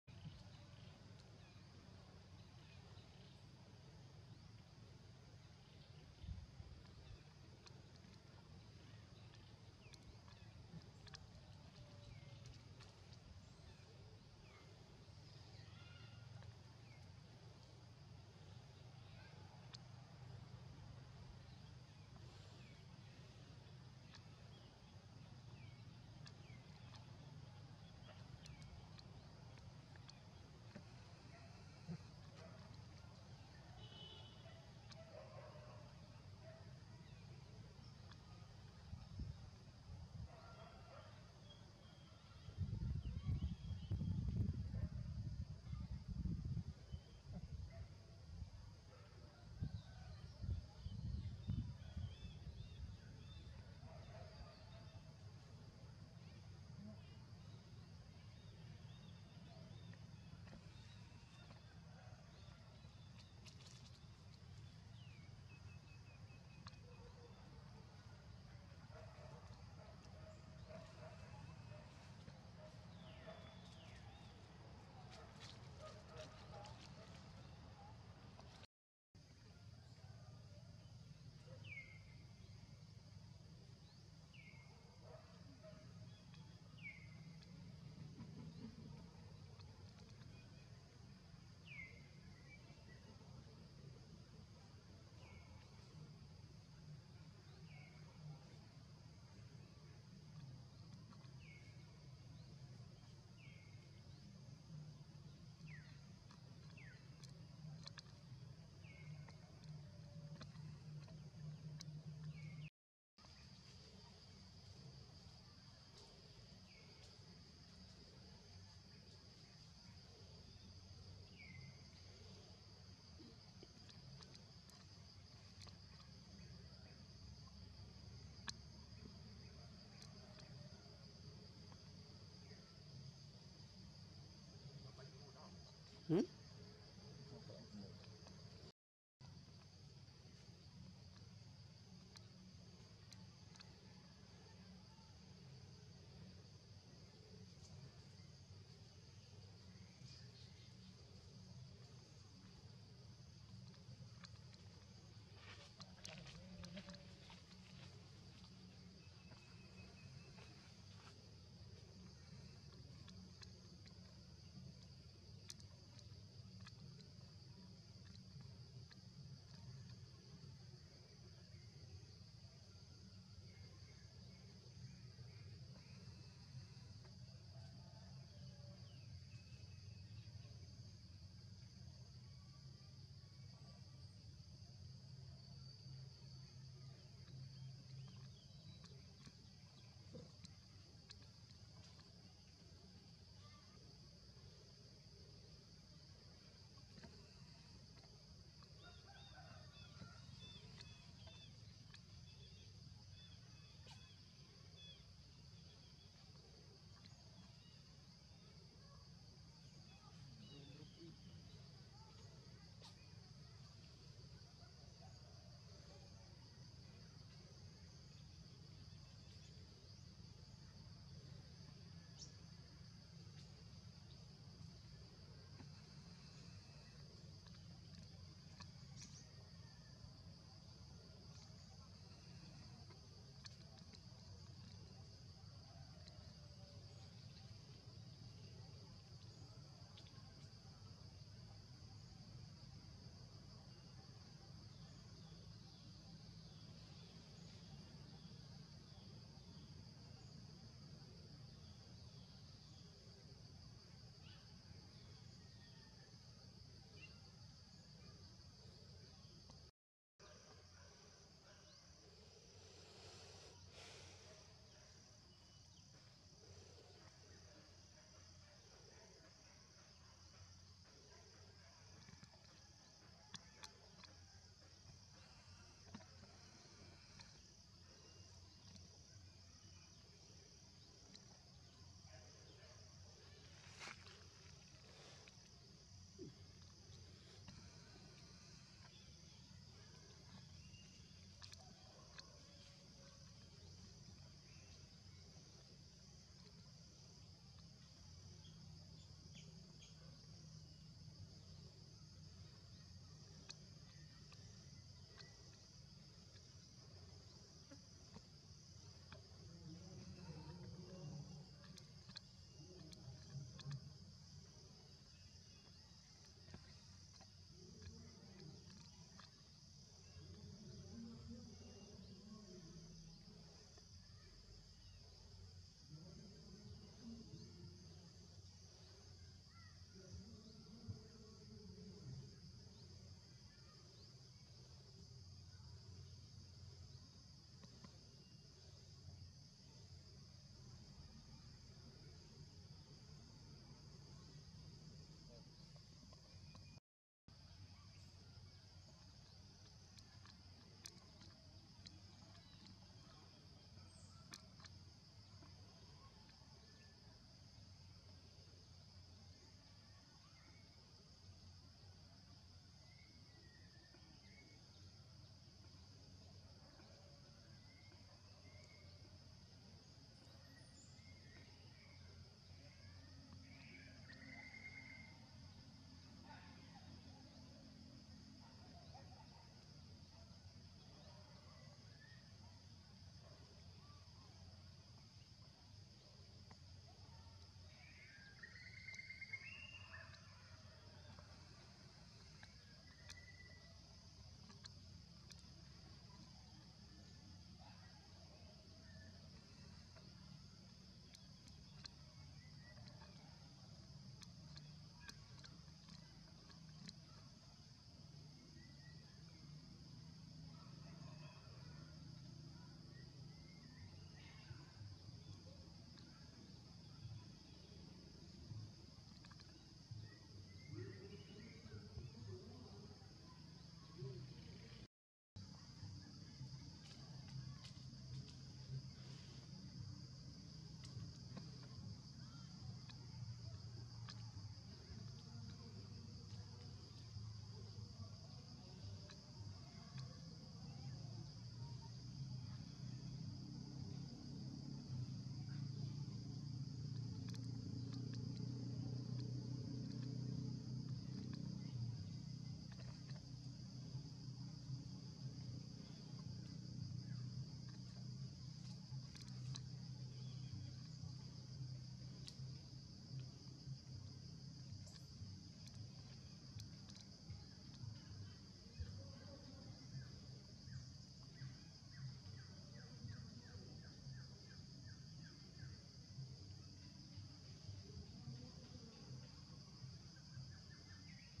They need to Find Safe Area Bec Amber Fallowing Behind
Amari troops.
May you have best safe my love. I am your behind line protector. Please escape quickly Bec Enemies fallowing us now. they walk on the top wall of Angkor Thom